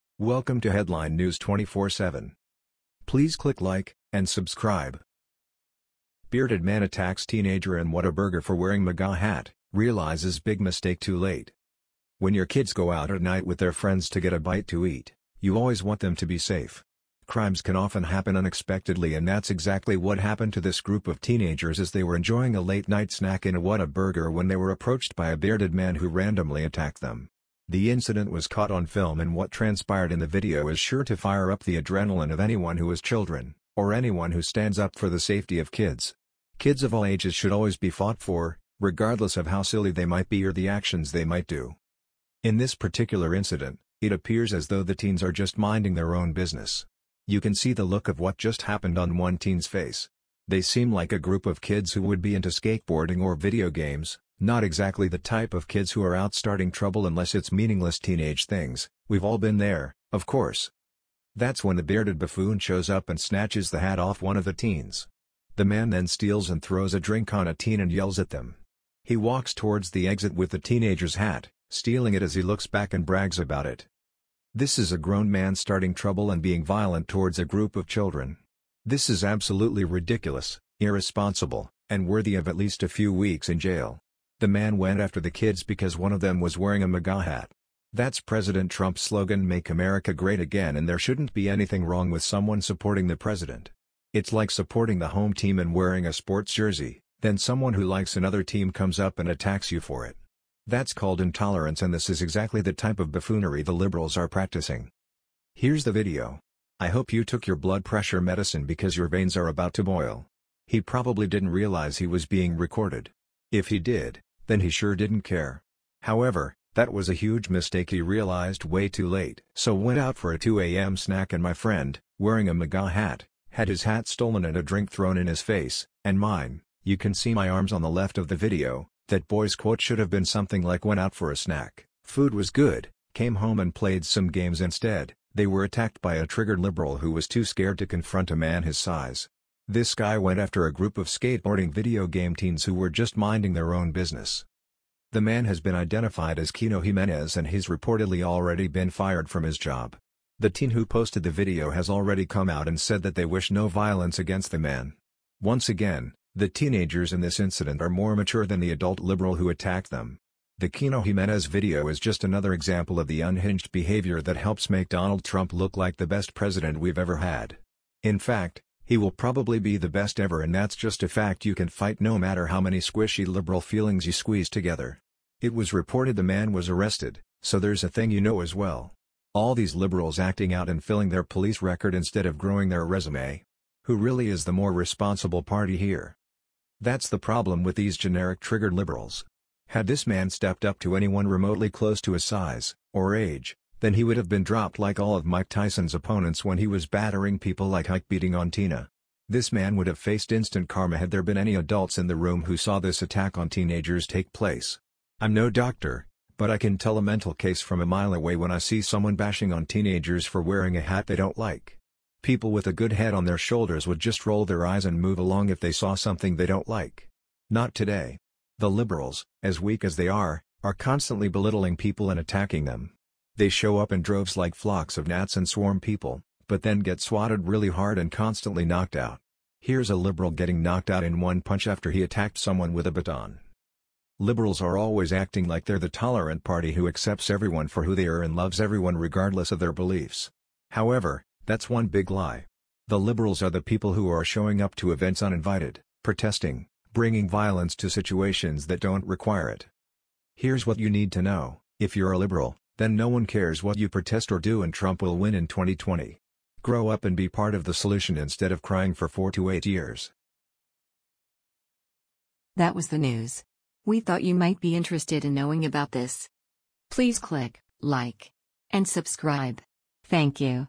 Bearded Man Attacks Teenager In What-A-Burger For Wearing MAGA Hat, Realizes Big Mistake Too Late
That’s when the bearded buffoon shows up and snatches the hat off one of the teens. The man then steals and throws a drink on a teen and yells at them. He walks towards the exit with the teenager’s hat, stealing it as he looks back and brags about it.